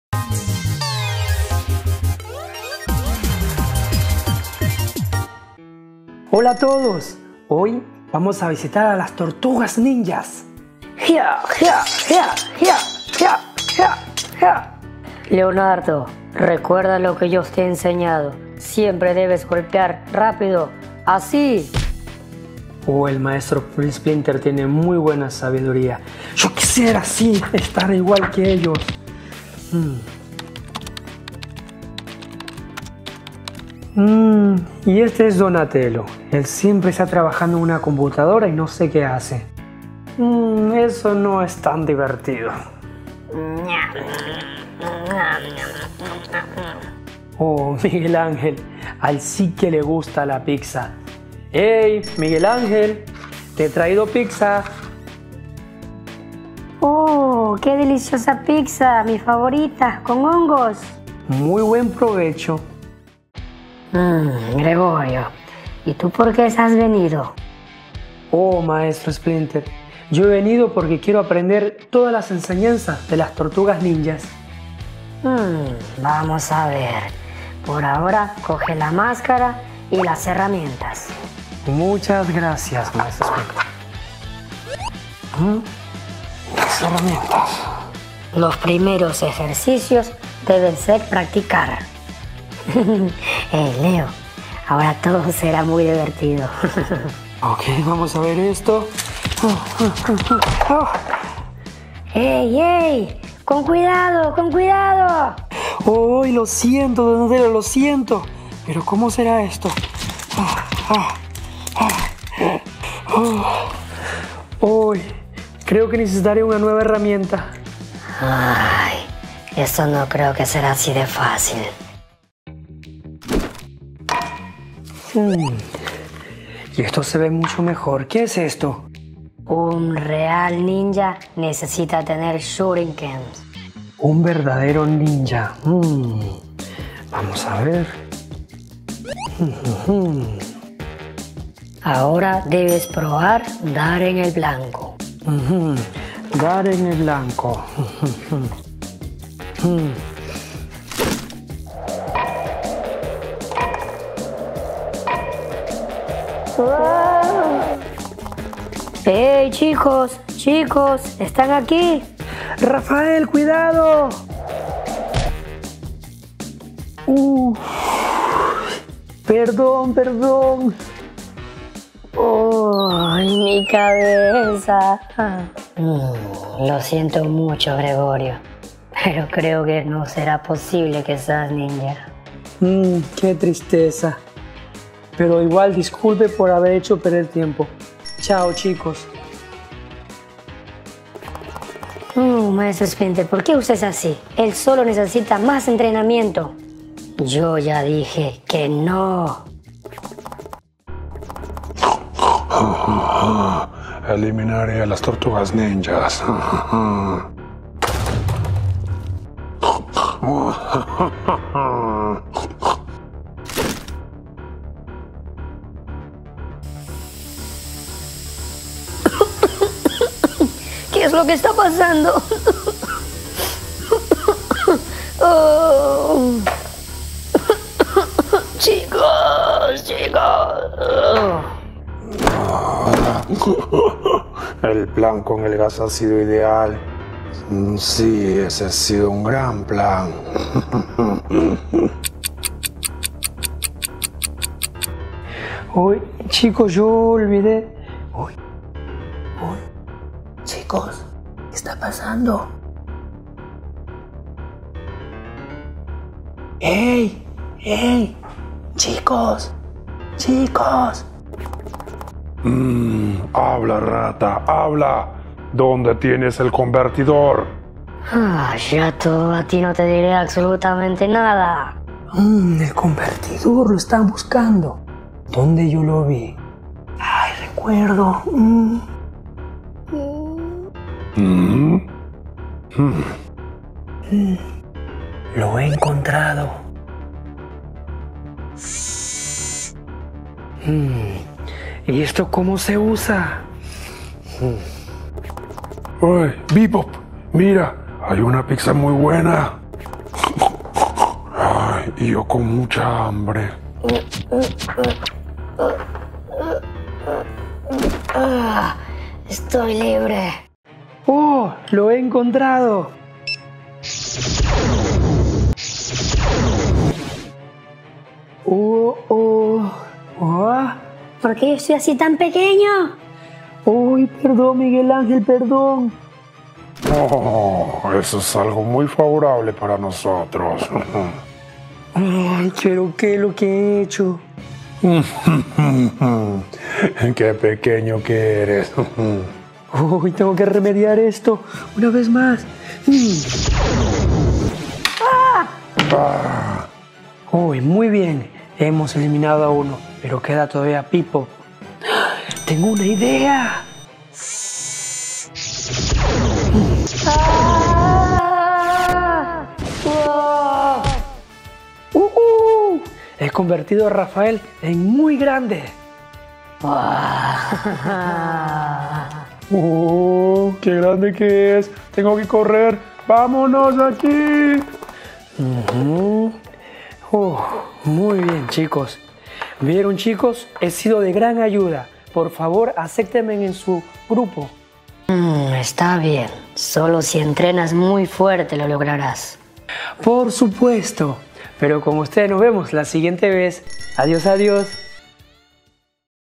¡Un entrenamiento con las Tortugas Ninja! Videos de juguetes para niños. Ninja Turtles español
Nuevos videos de juguetes Ninja Turtles español en el canal Caja Mágica.  Videos de juguetes para niños. Hoy Grigorio va a  aprender todas las enseñanzas de las Tortugas Ninja. ¡Gran idea! Master Sprinter lo entrenará. Grigorio  necesita ponerse una máscara y aprender a usar las herramientas. ¡Pongámonos a trabajar!

¡Juega con los  juguetes más populares!